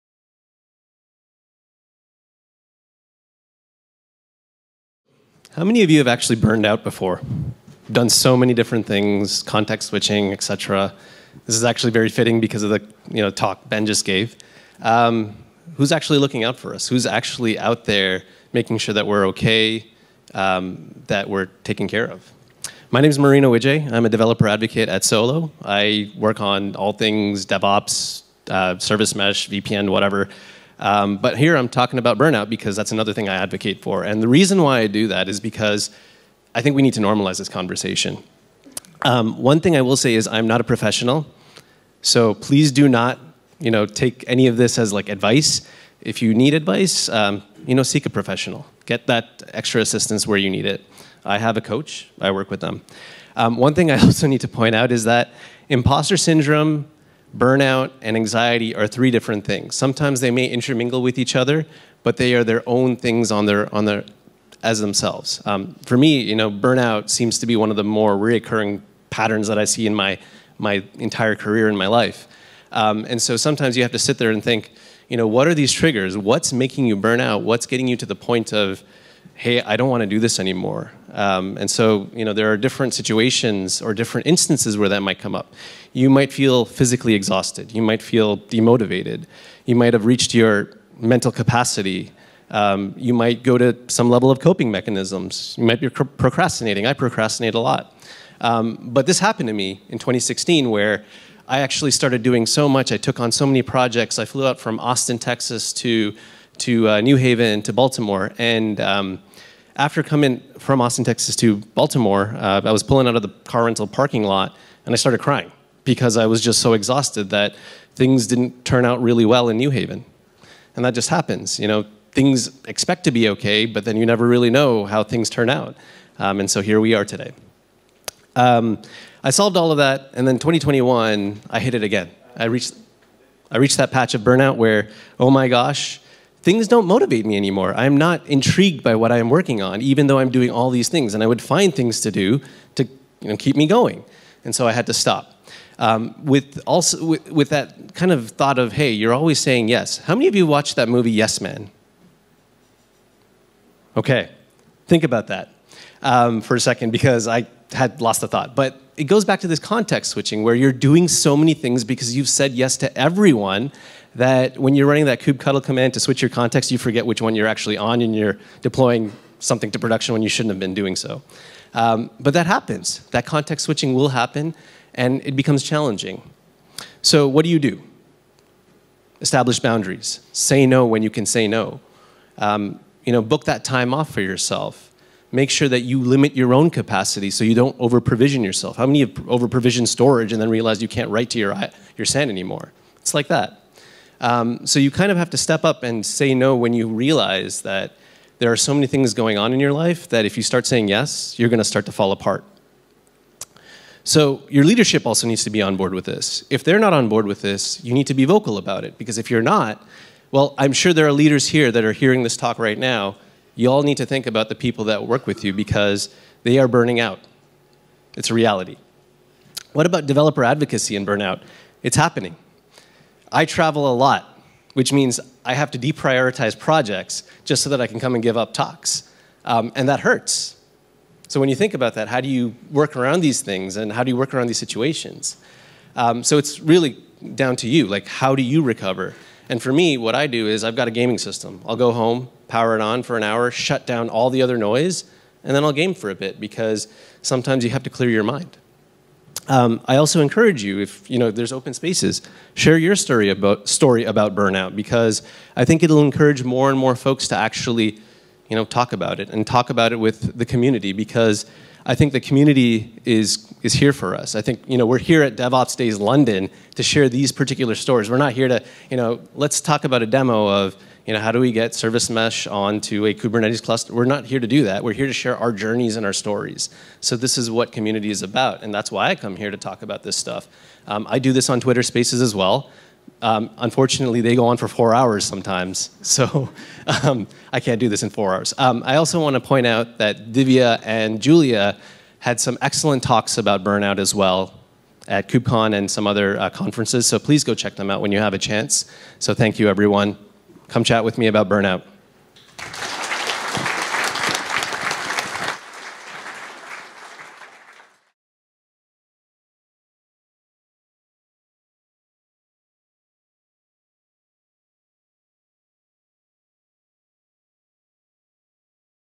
Marino Wijay - Who’s looking out for us?! A cautionary tale of BURNOUT and Recovery
This is video is from the DevOpsDays London conference, held at the Oval on September 29th and 30th 2022. In this video, you'll hear from a top expert in the field of DevOps. We hope you enjoy the talk and take away valuable information that you can apply to your own work.

“You’re a ROCKSTAR” “AMAZING WORK” “WOW you must never sleep” “Can you take this on? Yes? Great!”

“When will you finish it?” “Are you done yet?” “What’s taking so long?”

Burnout is a slow-burn to slightly destructive tendencies. We strive so much to be our best selves, and deliver the best work we can. Sometimes we push ourselves to extremes we sometimes cannot even fathom but more importantly, we forget the consequences of what’s next… Burnout. What happens when we approach and arrive at burnout?

* physical exhaustion
* mental capacity has been reached
* sleeping and eating patterns change
* lack of motivation and drive
* isolation, feeling alone, and lost
* procrastination
* coping mechanisms
* self-doubt, lack of self-worthiness
* feeling defeated

Allow me take you through a story of immense burnout and how I recovered, and then proceeded to face burnout again and decided I needed to make a change for the better.

Marino leads the Developer Relations and Advocacy team at Solo.io. He is passionate about technology and modern distributed systems. He will always fall back to the patterns of Networking and the ways of the OSI. Community building is his driving force; A modern Jedi Academy.